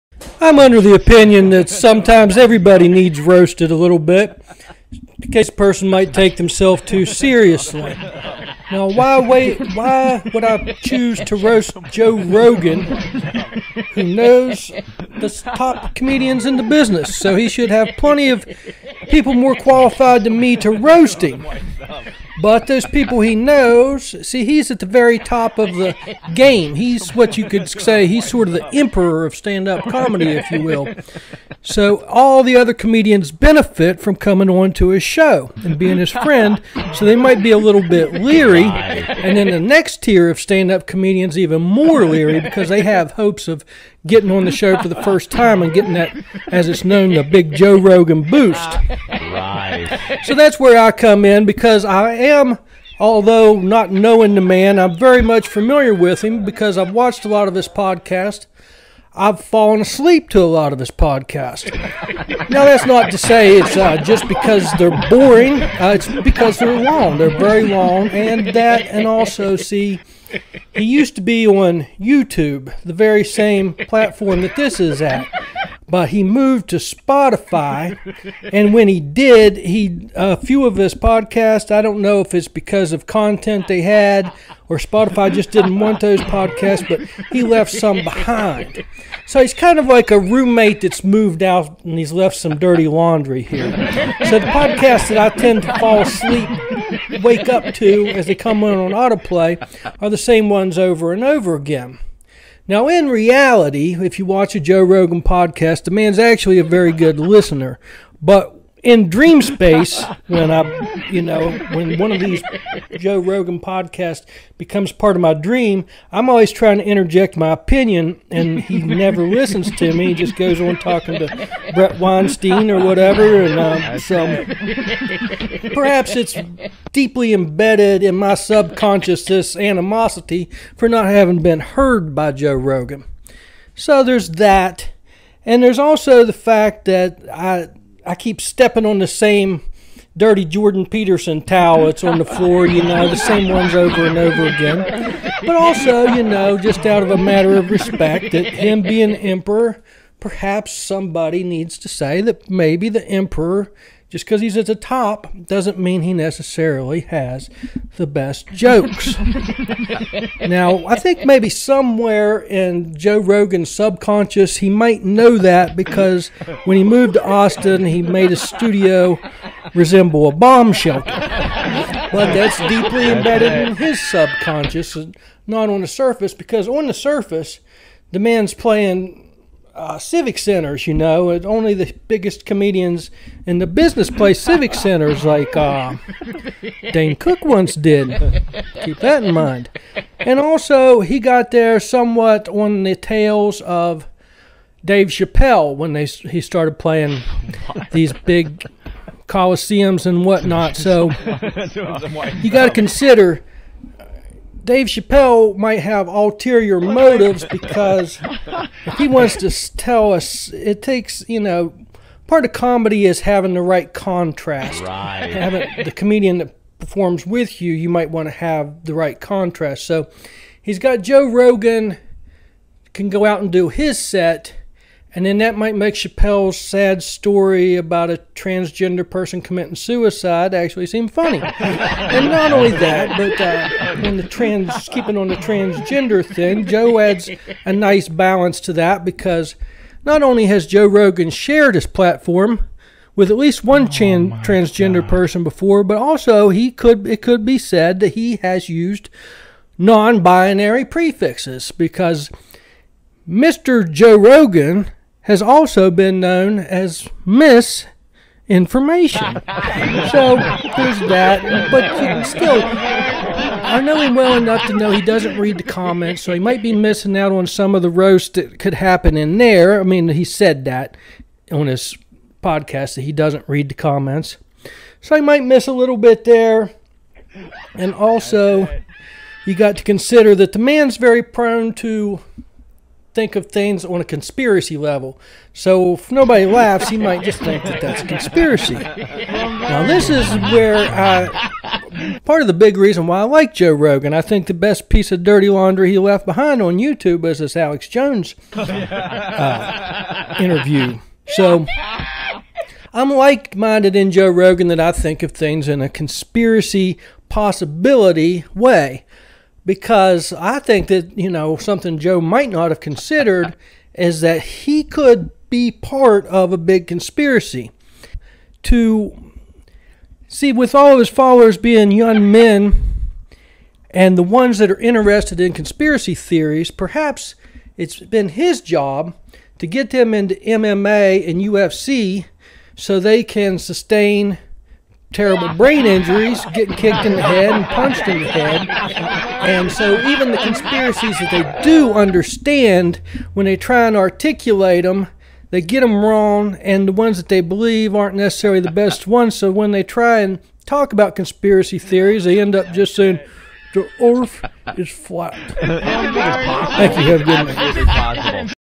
The Roast of Joe Rogan (Bob Burton stand-up 2022-07-30)
Legendary Podcaster, Comic, Actor, and MMA Commentator; Joe Rogan, Gets Roasted